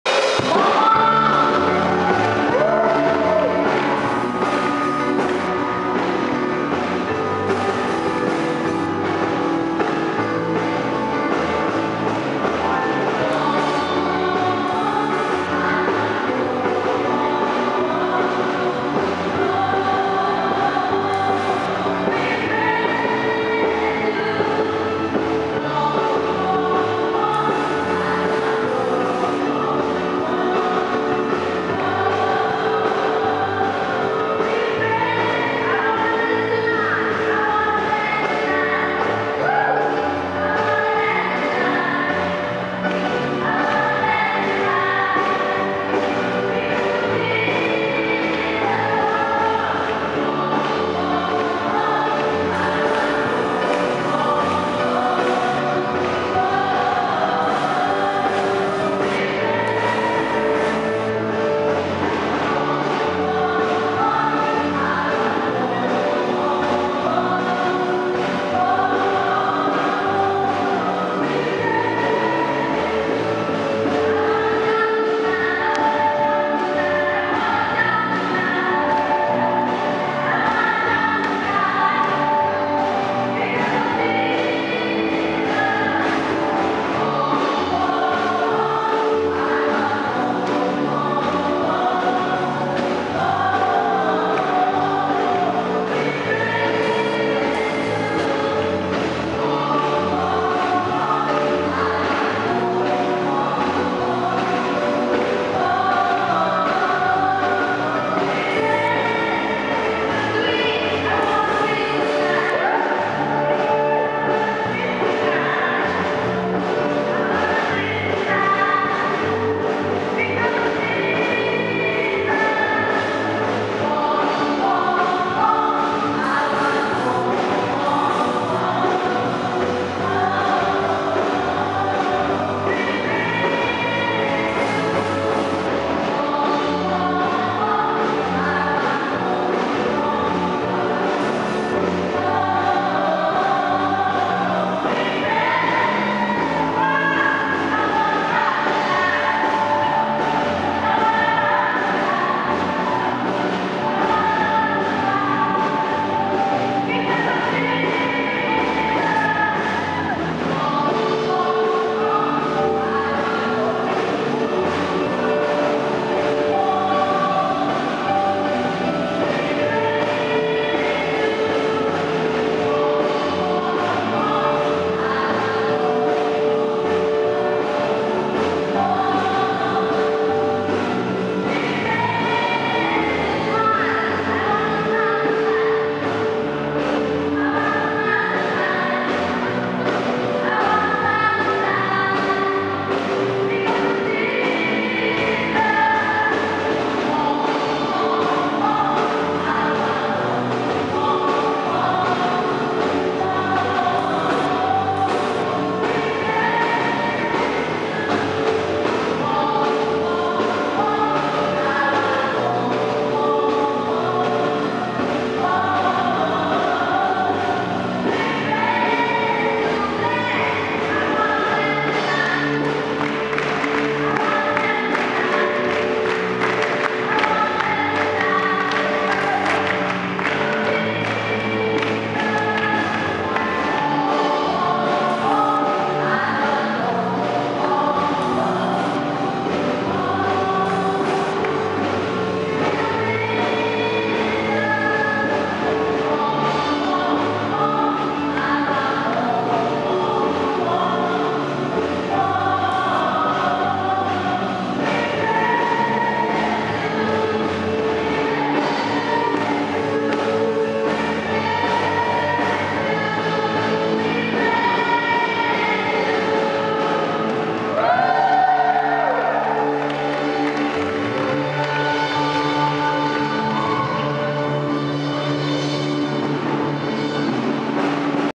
More more more!!!
Kem Transformasi Kudat. 7-10 Jun 2012. Kebaktian Malam yg terakhir. Dan pada 1.00 pagi mereka masih semangat menyanyi dan menari bagi Tuhan! Keterobosan!!!